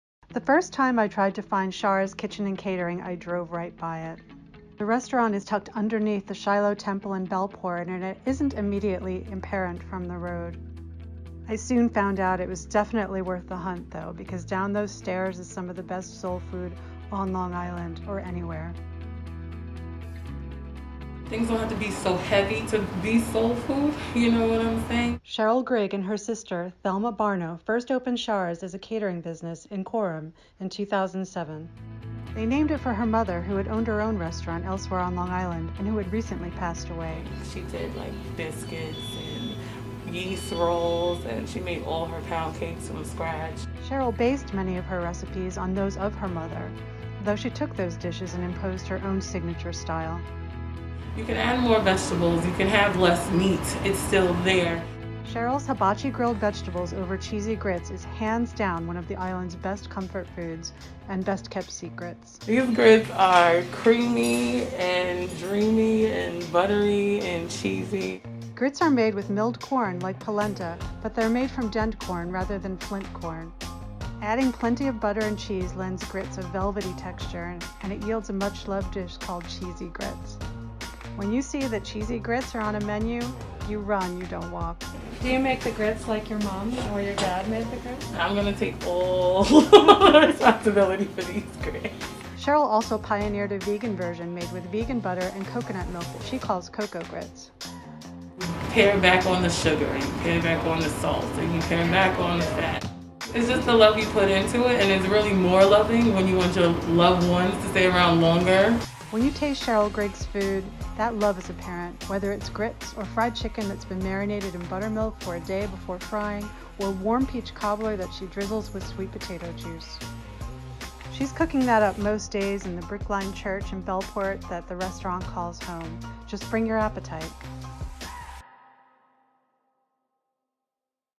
Eatery hidden in church serves some of LI's best soul food
Cheryl Griggs and her sister, Thelma Barno, opened Chara's Kitchen and Catering in Bellport in honor of their late mother who once owned her own soul food restaurant. Newsday food critic Corin Hirsch takes us to the restaurant to have a look at Cheryl's healthy spin on a classic soul food dish. (Credit: Newsday / Raychel Brightman; Photo credit: Cheryl Griggs)